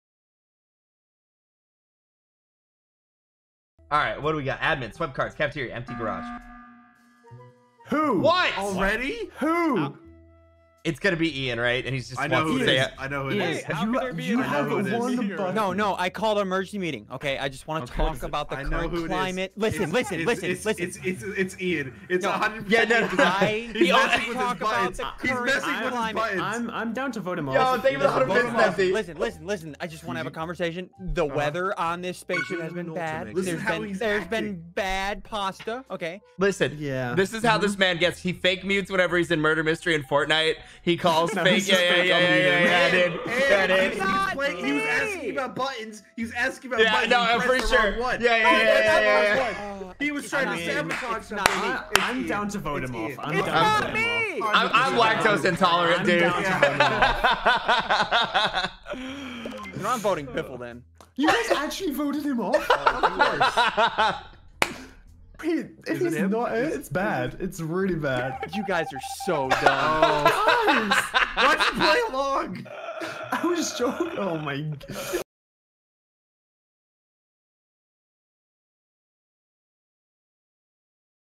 SSundee gets voted off for pressing button for a dumb reason
Credits
All the credit for this short video goes to Sigils